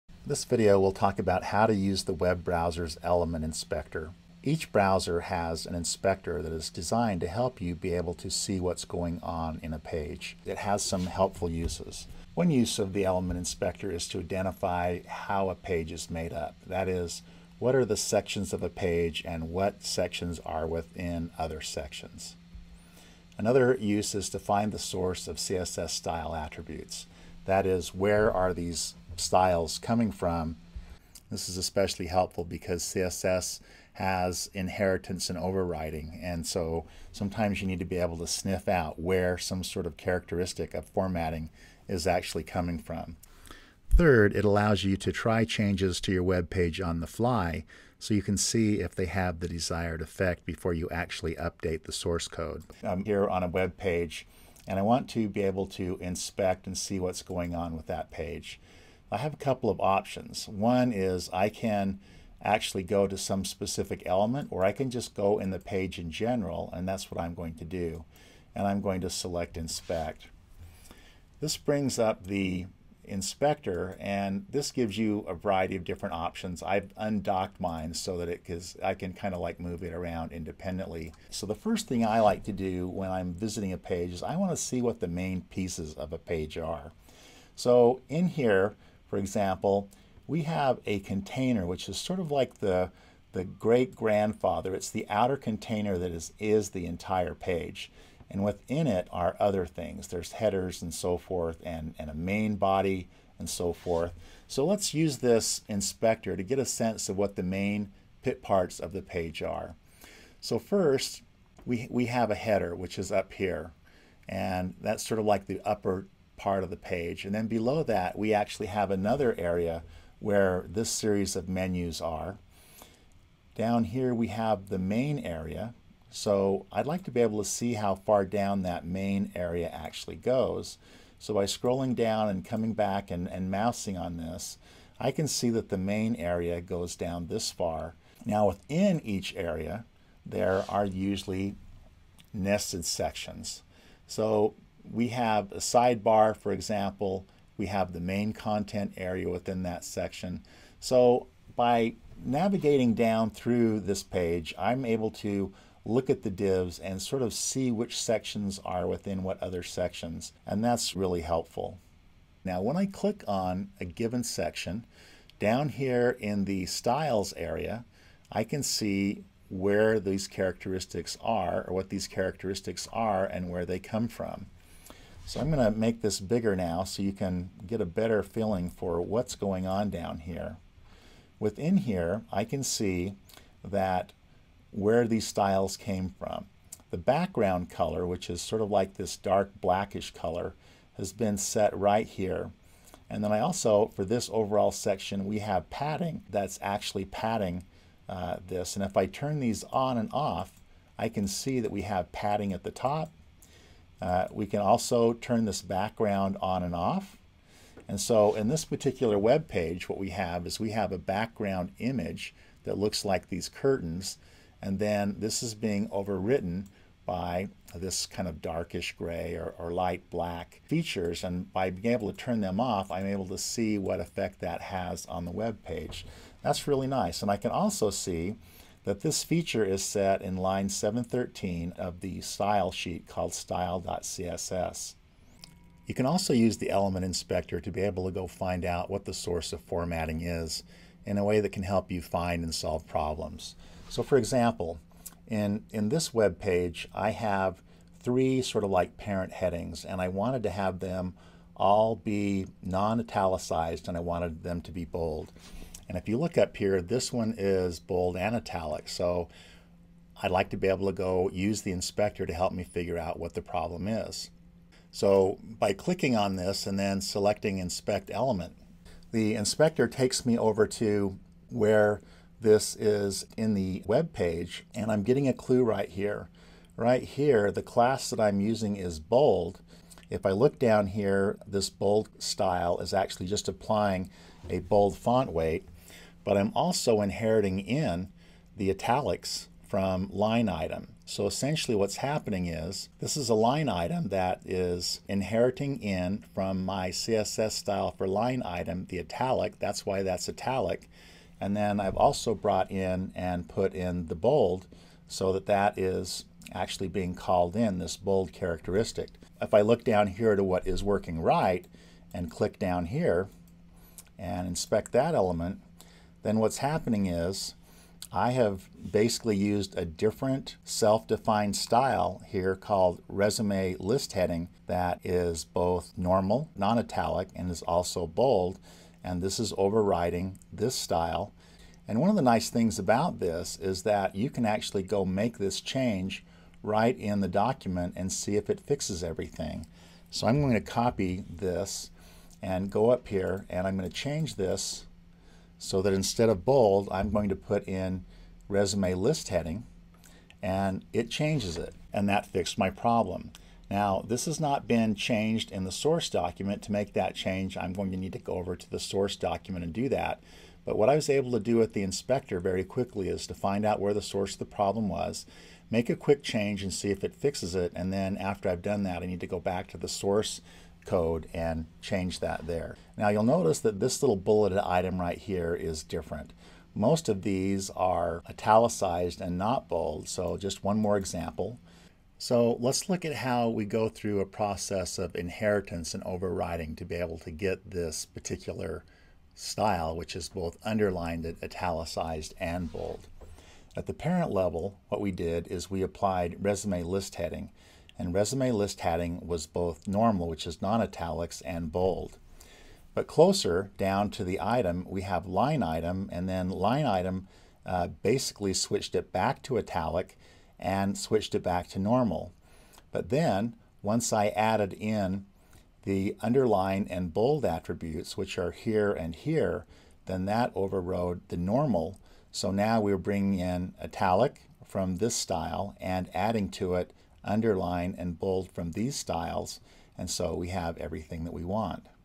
Using the browser inspector
Using the web browser inspector to see the components of a web page and where the CSS style attributes come from. The element inspector also can serve as a sandbox. That is, it lets you try changes in the inspector to see if the changes has the desired effect on the page. If so you can make the change in the source code outside of the inspector.